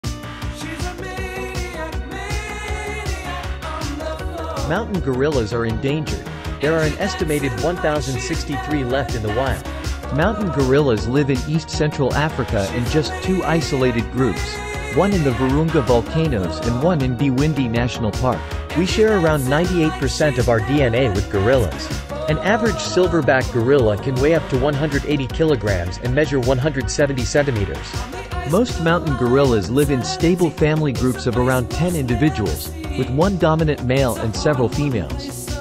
TOP 5 Jaw-Dropping Gorilla Facts You Won't Believe!
Prepare to be amazed as we uncover the top 5 mind-blowing facts about gorillas! From their incredible strength to their complex social behaviors, these astonishing primates will leave you in awe. Dive into the fascinating world of gorillas and discover secrets you won't believe! 🦍💡